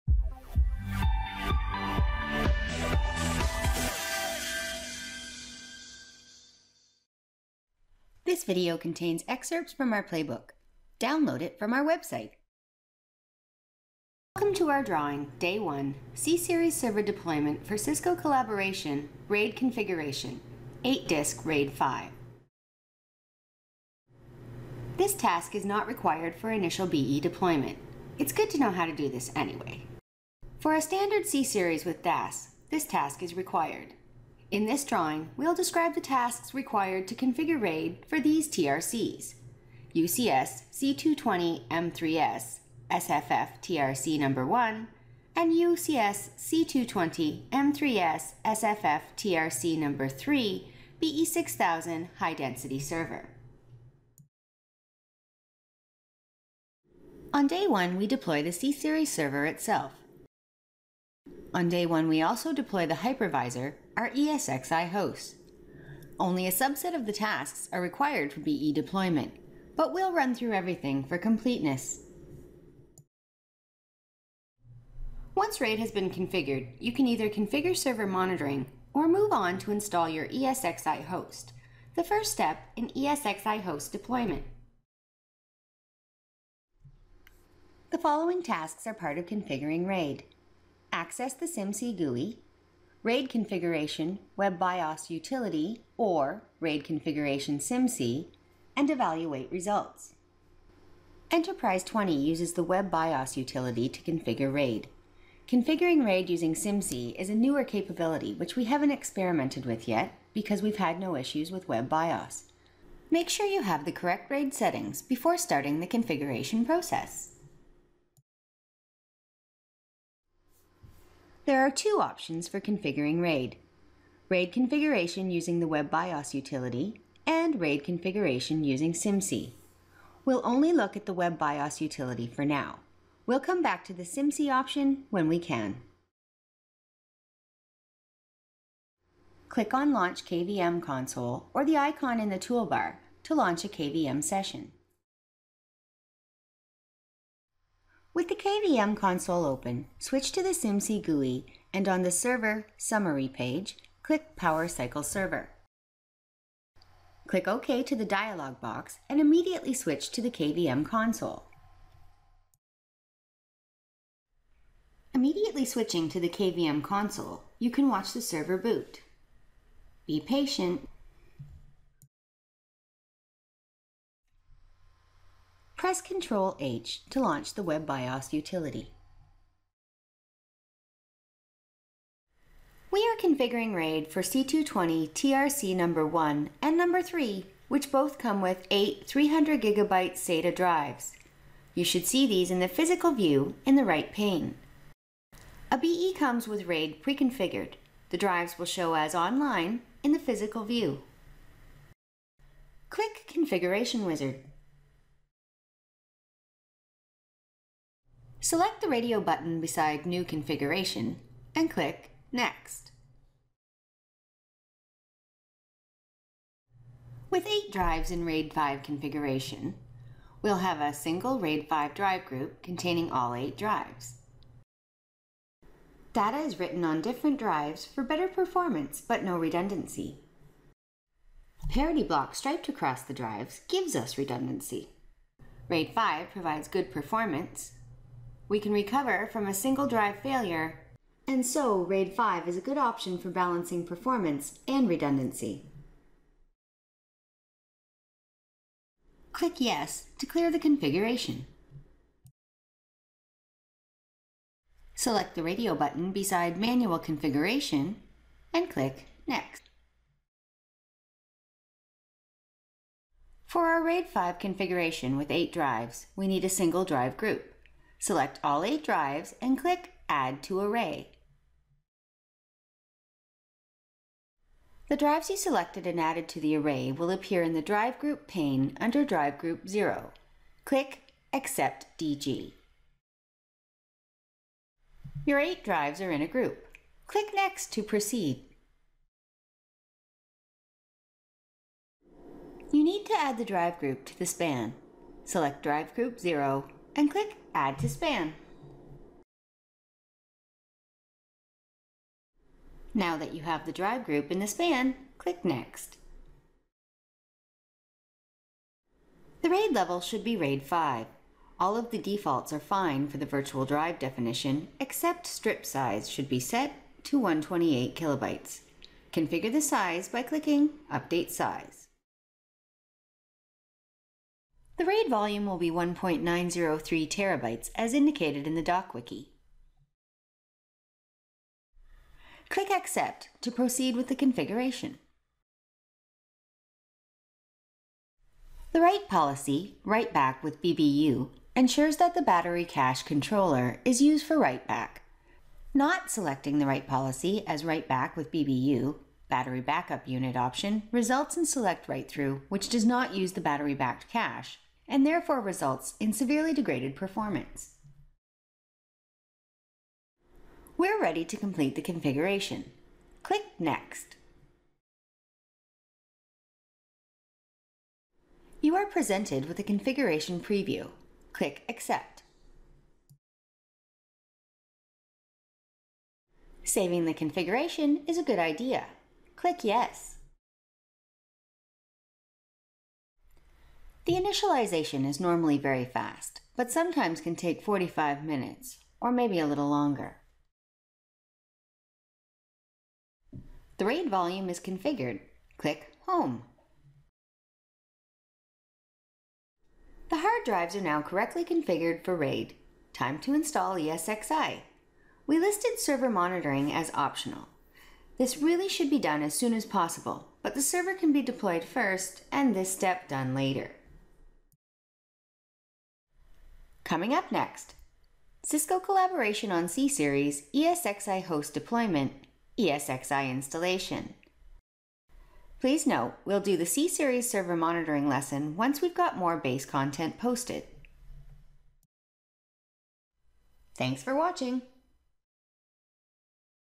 Day 1: UC on C-Series 8-Disk RAID 5 for C220M3 TRC #1 and #3
This drawing looks at RAID 5 configuration for UCS C220 M3S (SFF) TRC#1 and UCS C220 M3S (SFF) TRC#3 (BE6000 "High Density (HD)" Server). RAID configuration for a BE is not required as the server comes with RAID configured and ESXi installed. It's good to know how to do this regardless.